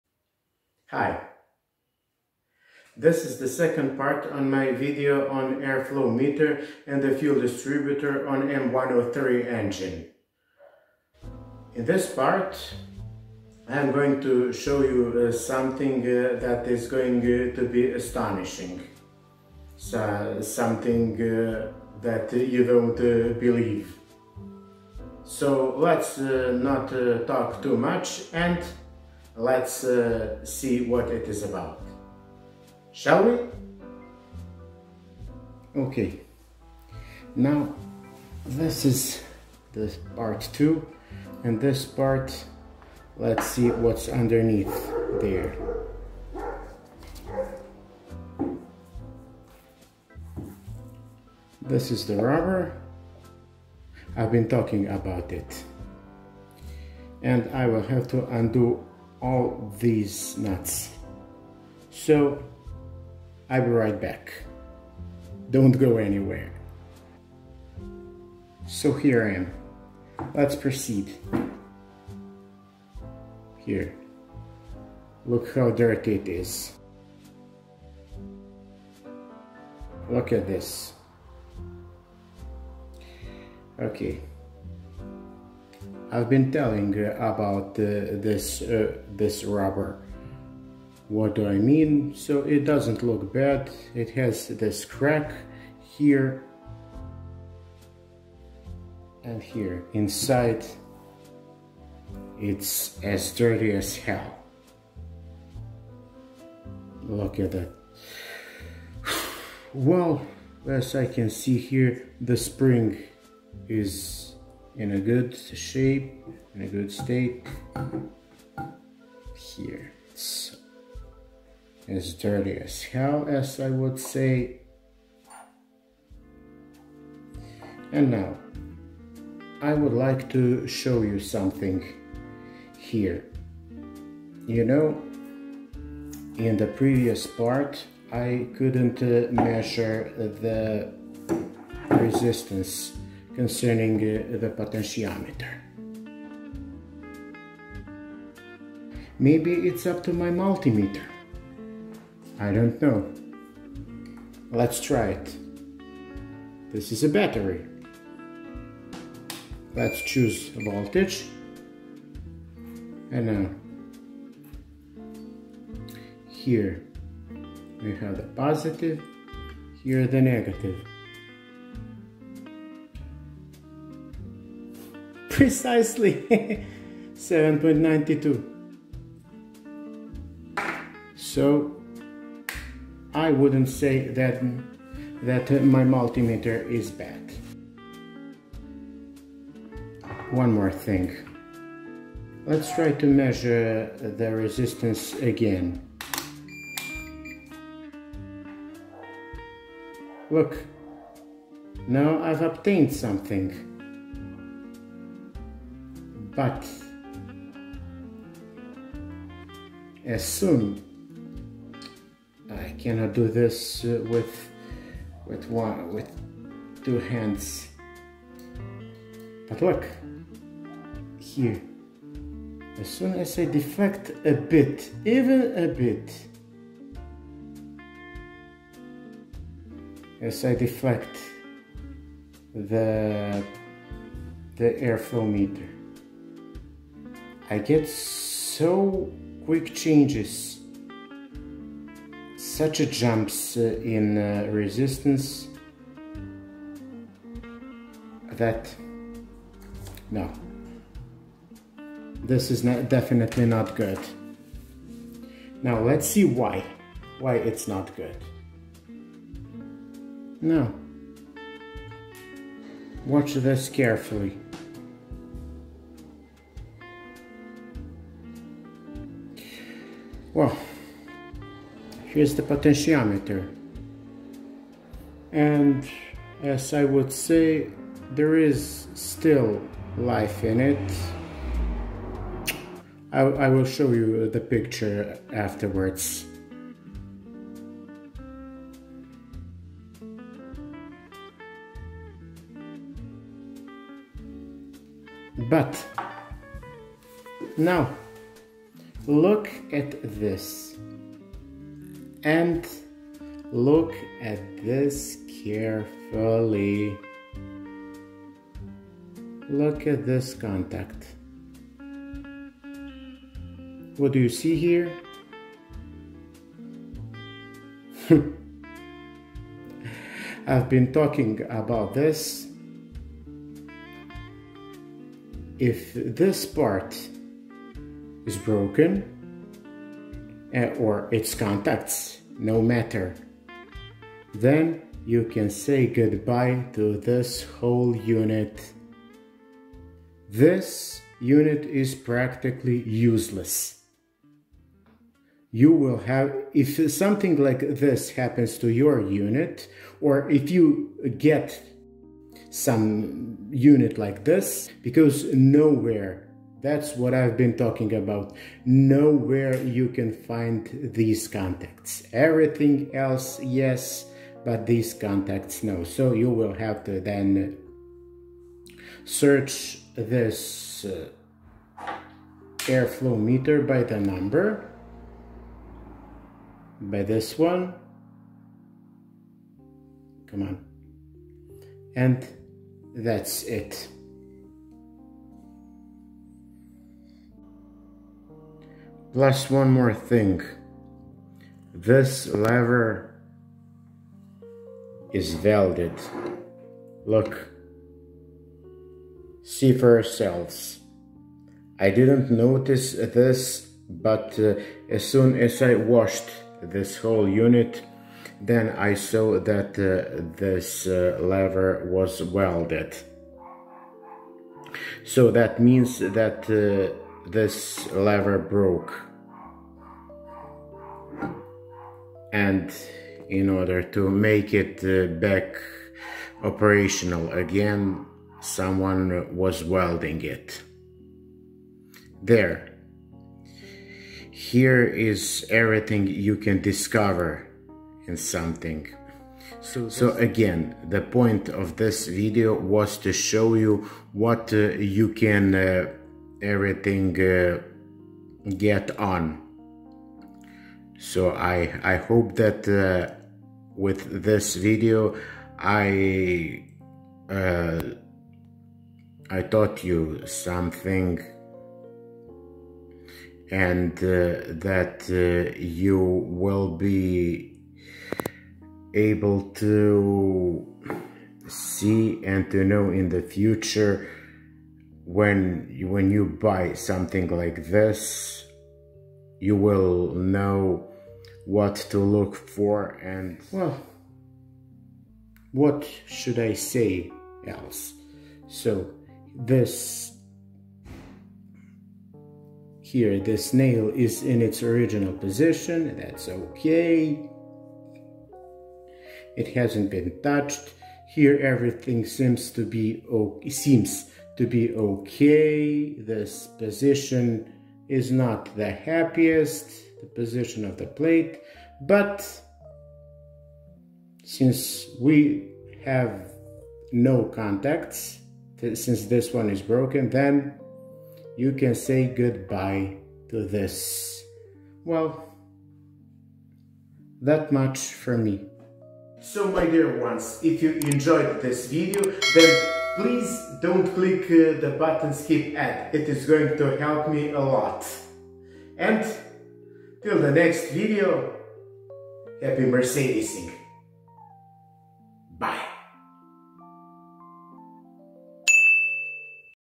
㉦Mercedes Ke-Jetronic㉦ - M103 Air flow meter in details
For all of you to understand the system in brief - Developed in 1973 and being mounted into cars up to 1993 as it went in history. But why these to letters!?(KE) K(from German "kontinuierlich" meaning continuos, without interruption) the MECHANICAL injection system on which the fuel starts flowing from the fuel tank sucked by the FUEL PUMP and delivered to the FUEL DISTRIBUTOR which sends fuel to the INJECTORS while the majority of it flows through the FUEL PRESSURE REGULATOR through its return line back to the fuel tank again. The letter E(electronic) has to mark the electronic sensors (the developement starting from early eighties) which had to inform the ECU about the current state of the engine so that the ECU(Electronic Control Unit) then could create current signal in mAmp(miliampers) and send it to four crucial components for the system - the EHA(the electro-hydraulic actuator,a unit consisting of a magnet, an electromagnet and a baffle plate) which had to fine tune the engine in each of its phases, the ICV(the idle control valve), the unit which was predicted for the engines perfect idle, the X11(the diagnostic socket) where the duty cycle(the ideal mixture) was adjusted and the X11/4(the code reader) - the socket which from any codes were read if present using an OBD1 scanner.
"Happy Mercedesing"